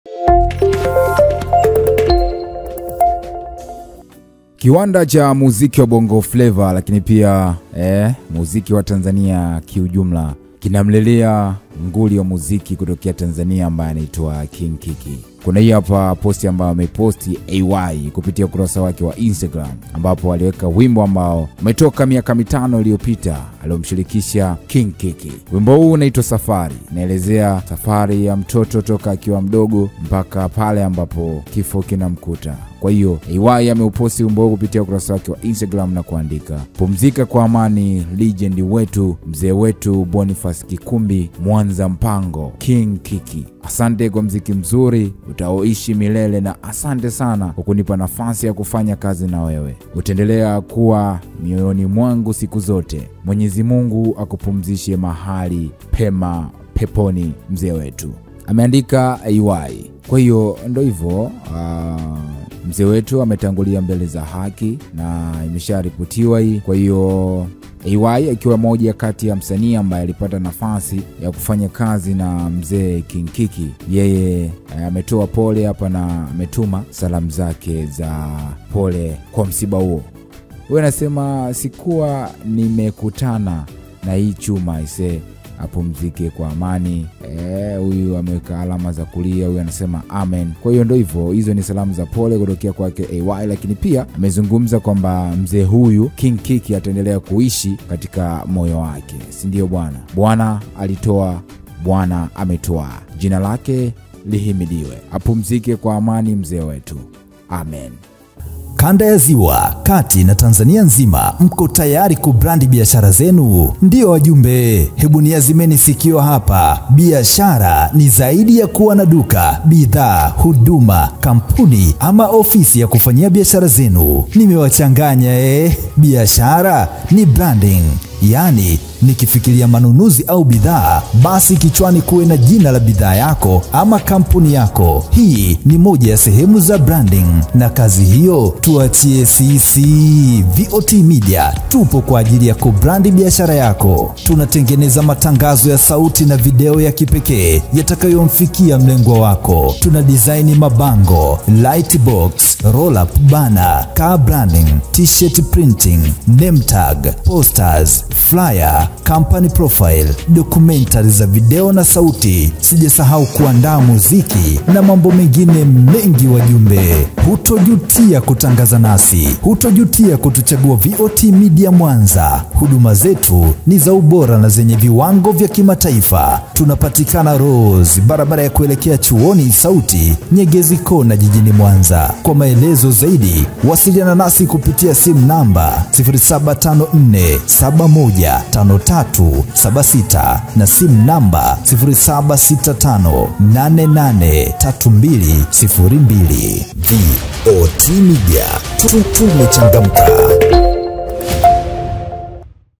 KIFO CHA KING KIKII, AY ALIA KWA UCHUNGU | ATAJA SABABU HII
MSANII AY ALIA KWA UCHUNGU KIFO CHA NGULI KING KIKII